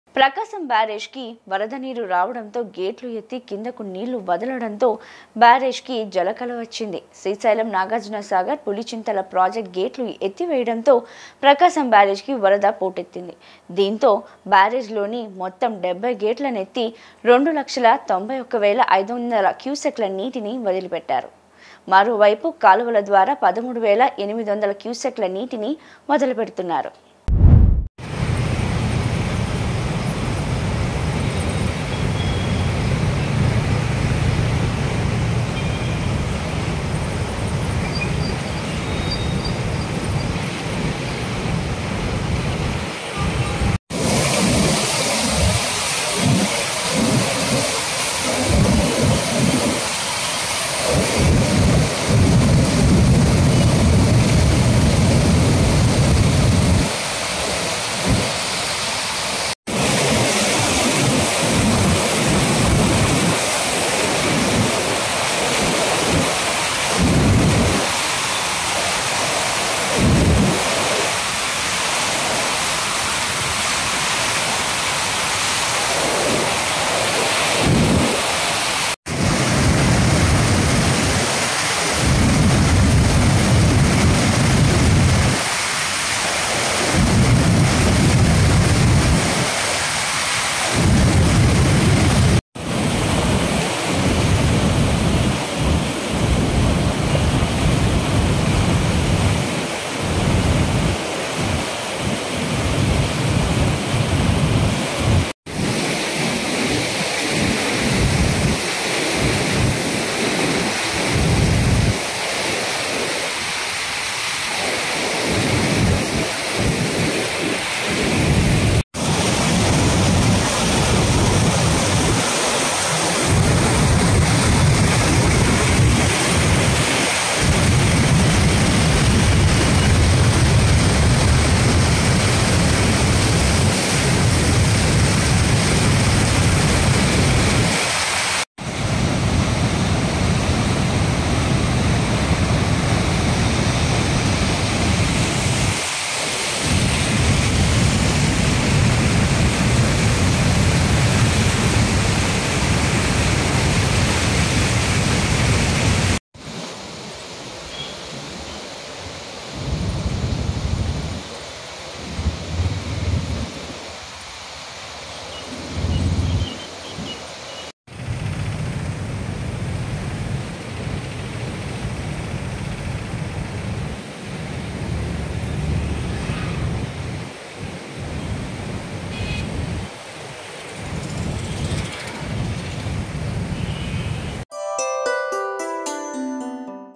ప్రకాశం బ్యారేజ్ కి వరద నీరు రావడంతో గేట్లు ఎత్తి కిందకు నీళ్లు వదలడంతో బ్యారేజీకి జలకళ |C News
ప్రకాశం బ్యారేజ్ కి వరద నీరు రావడంతో గేట్లు ఎత్తి కిందకు నీళ్లు వదలడంతో బ్యారేజీకి జలకళ వచ్చింది. శ్రీశైలం నాగార్జునసాగర్ పులిచింతల ప్రాజెక్టు గేట్లు ఎత్తివేయడంతో ప్రకాశం బ్యారేజీకి వరద పోటెత్తింది. దీంతో బ్యారేజీలోని మొత్తం 70 గేట్ల నెత్తి 2లక్షల 91,500 క్యూసెక్కుల నీటిని విడిచిపెట్టారు. మరోవైపు కాలువల ద్వారా 13,800 క్యూసెక్కుల నీటిని విడిచి పెడుతున్నారు